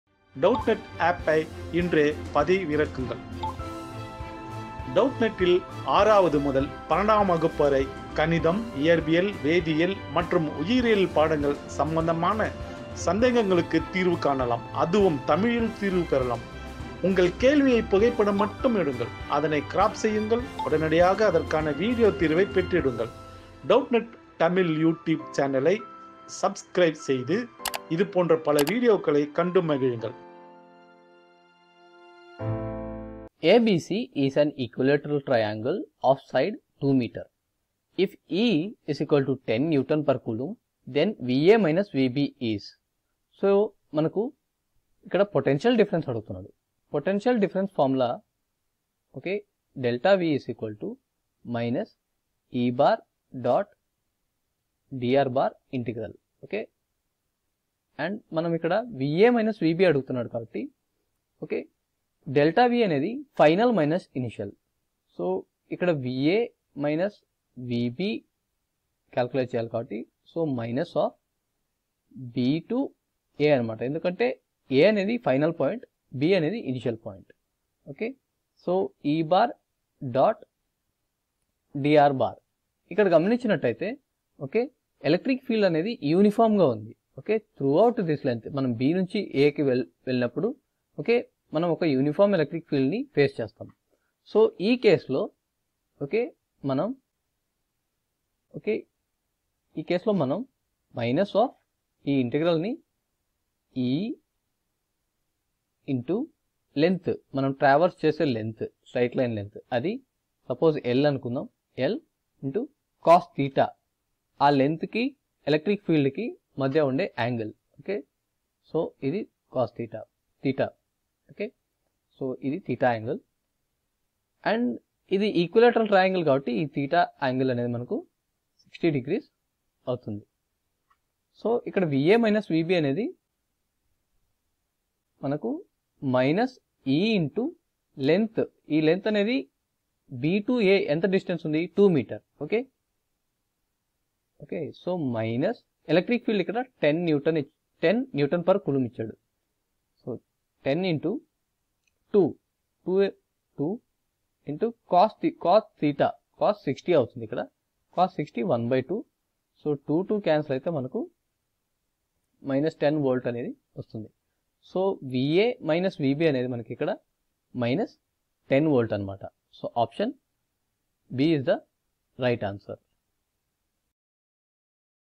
ABC is an equilateral triangle of side 2m. If `vec(E ) = 10 NC^(-1)` | Class 12 Physics | Doubtnut
ABC is an equilateral triangle of side 2m. If `vec(E ) = 10 NC^(-1)` then `V_(A) - V_(B)` is br img src=httpsdoubtnut-static.s.llnwi.netstaticphysics_imagesAKS_TRG_AO_PHY_XII_V02_A_C01_E02_098_Q01.png width=80%

Online Class में Admission लेने के लिए यहाँ click करें :
👉Class 12 | CBSE Boards | English Medium :

👉 Have Any Query? Ask Us.